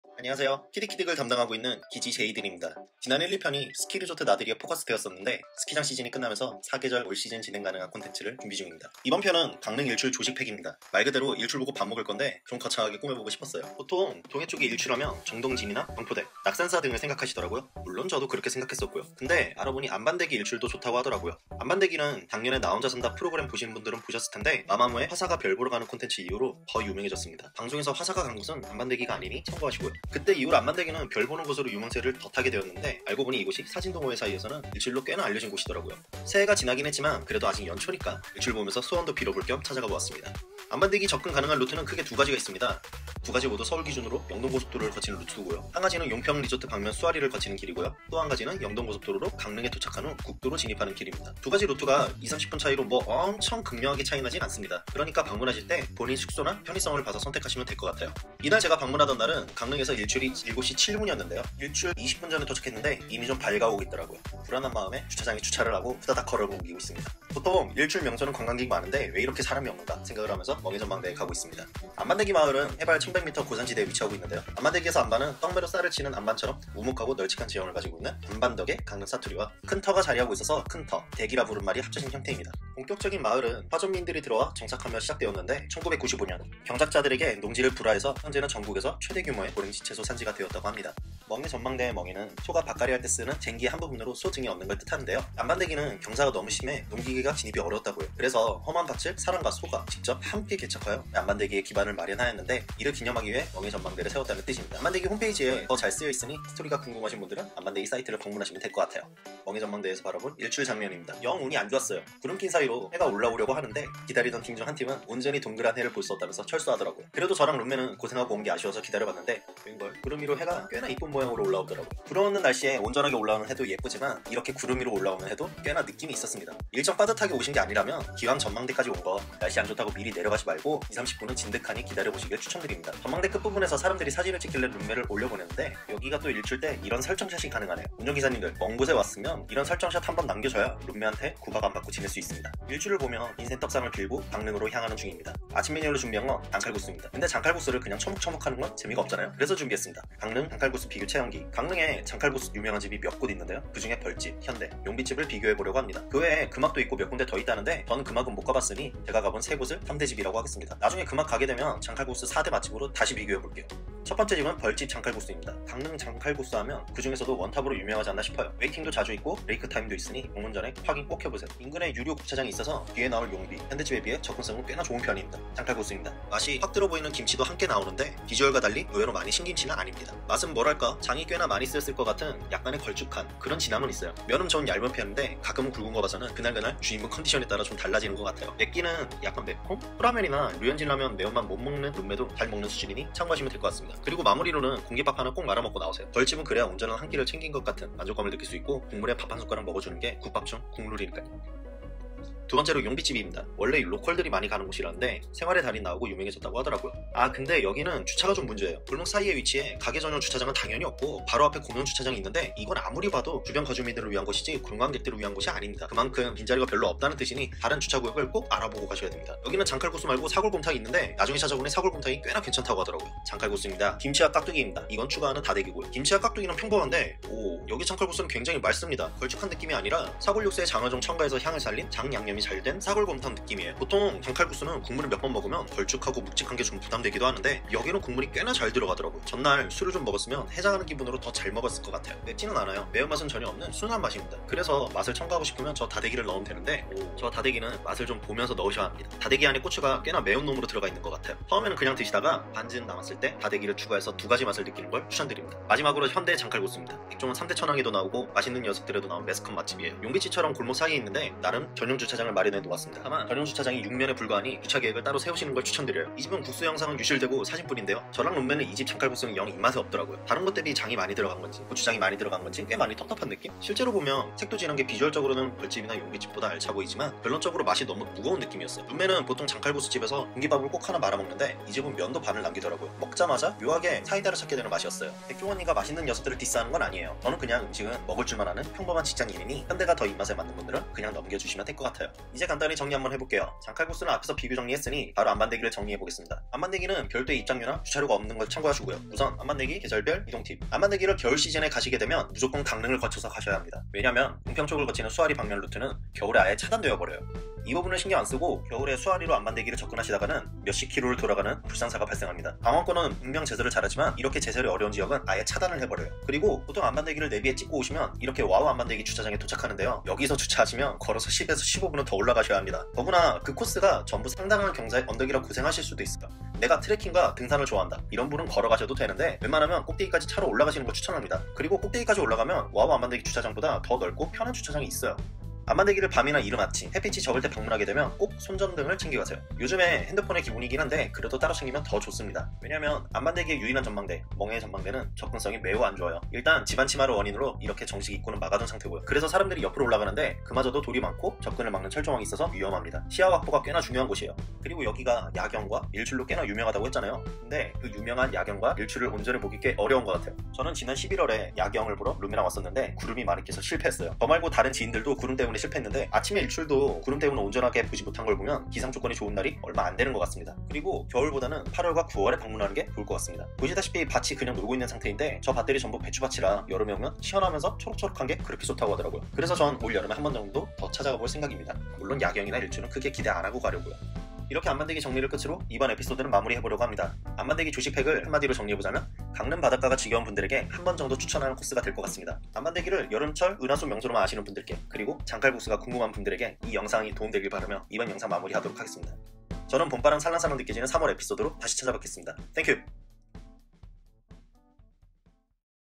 [KI득키득] KIJI인의 내돈내산 리뷰 - EP. 3 강릉 일출 조식 PACK
*편집 이슈로 음향이 다소 좋지 않습니다 😥

‘KI득키득’ 세번째 이야기! 👏👏👏
강릉 일출 나들이를 위한
강릉 일출 조식 PACK !

지난 1-2편 리조트 콘텐츠는 겨울에만 즐길수 있는 제한적인 장소였는데요,
그래서 이번에는 1년 365일 언제나 즐길 수 있는 해돋이를 아이템으로 나들이를 다녀왔습니다.🚗

여름밤 은하수로 잘 알려졌지만, 의외로 일출 포인트였던 안반데기
그리고 선택장애를 가지신 분들을 위한 강릉 장칼국수 3대 맛집 비교까지

일출하면 정동진/경포대/낙산사만 떠올리게되는 분들에게 추천합니다 !

안반데기 위치 : 강원 강릉시 왕산면 안반덕길 428

벌집 장칼국수
주소 : 강원 강릉시 경강로2069번길 1
오픈시간 : 10:30 ~ 18:30
                   (재소소진시 off / 15:00 ~ 17:00 브레이크타임 / 화요일 휴무)
장칼국수 : 7,000원
주차정보 : 인근 유료주차장 이용 가능 (도보 3분 내)

용비집
주소 : 강원 강릉시 남문길 20
오픈시간 : 10:00 ~ 16:00 (화요일 휴무)
장칼국수 : 6,000원
주차정보 : 바로 앞 공영주차장 있으나 빈공간 찾기 어려움

현대장칼국수
주소 : 강원 강릉시 임영로182번길 7-1 할머니현대장칼국수
오픈시간 : 10:00 ~ 19:00 (화요일 휴무)
장칼국수 : 7,000원
주차정보 : 전용주차공간 6면

*안반데기 마을 통행 관련하여 안반데기 사이트를 참고하세요
 (코로나 이슈로 변동사항이 발생할 수 있습니다)